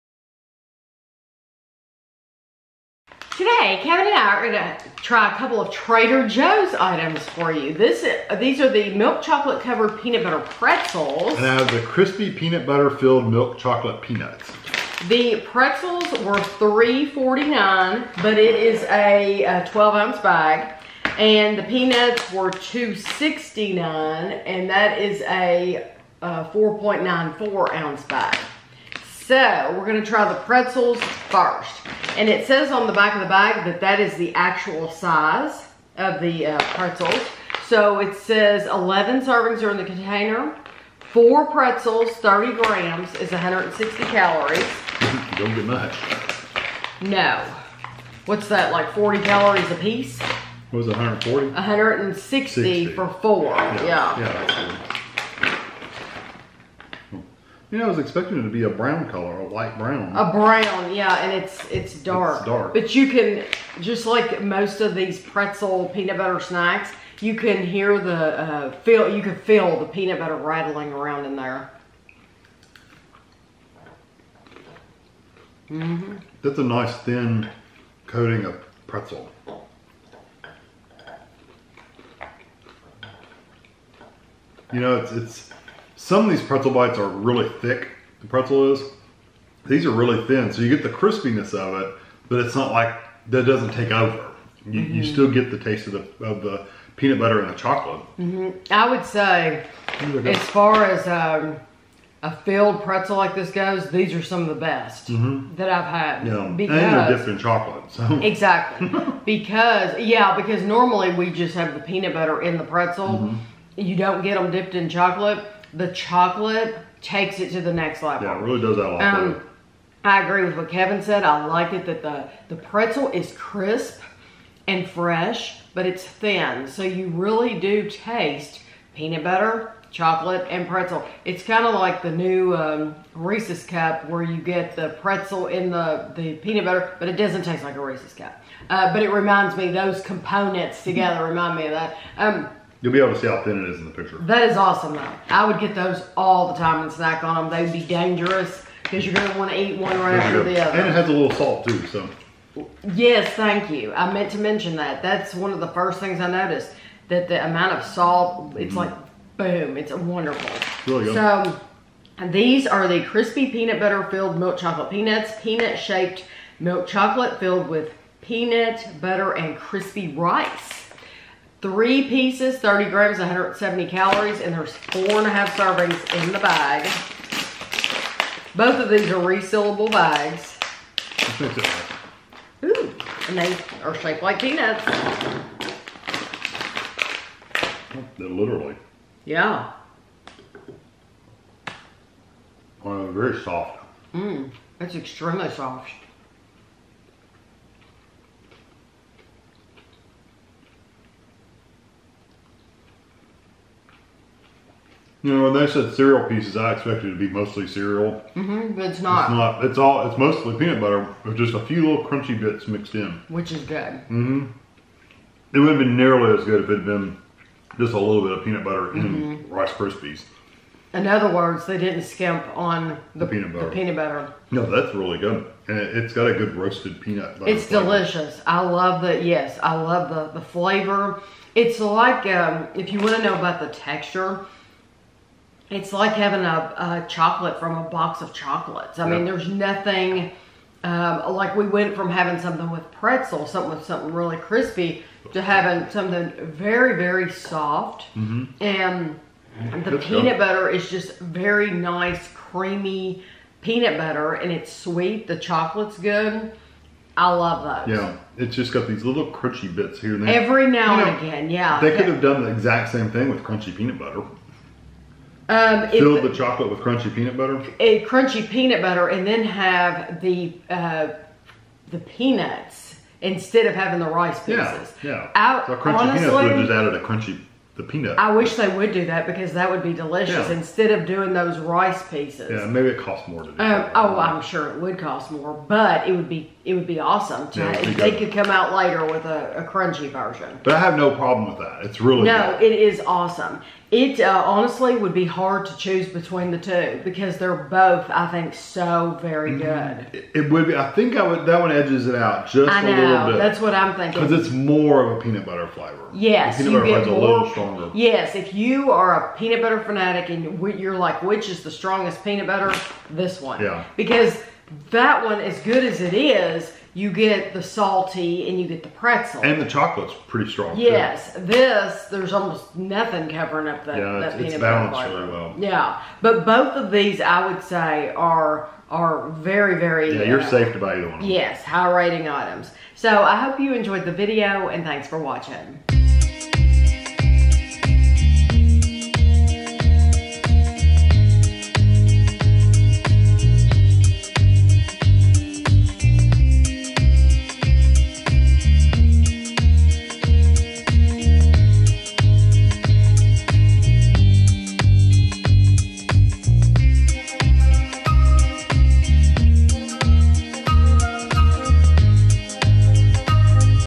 Trader Joe’s: Crispy PB Filled Milk Chocolate Peanuts & Milk Chocolate Covered PB Pretzels Review
This is a taste test/review of the Trader Joe’s Crispy Peanut Butter Filled Milk Chocolate Peanuts and Milk Chocolate Covered Peanut Butter Pretzels.
* The Crispy Peanut Butter Filled Milk Chocolate Peanuts were $2.69. 3 pieces (30g) = 170 calories

* The Milk Chocolate Covered Peanut Butter Pretzels were $3.49. 4 pretzels (30g) = 160 calories